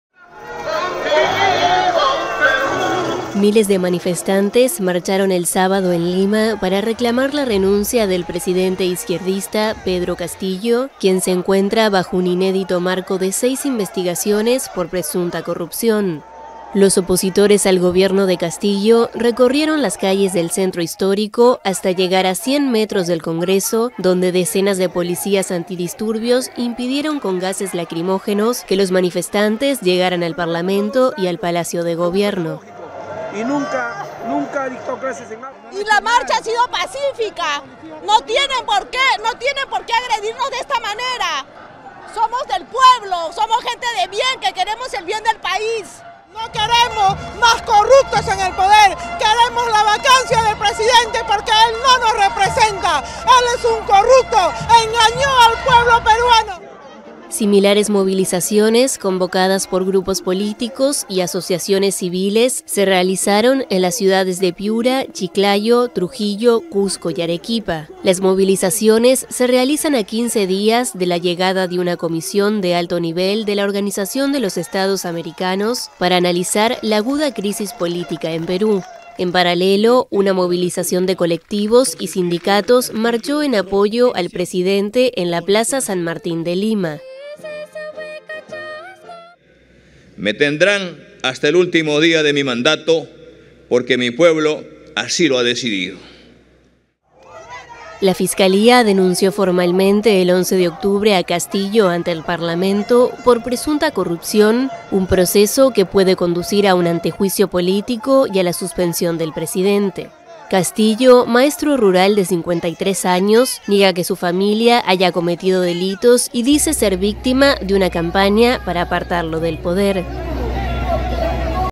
Noticias12Nicaragua Miles marchan en calles de lima contra presidente peruano Castillo
Miles marchan en calles de lima contra presidente peruano Castillo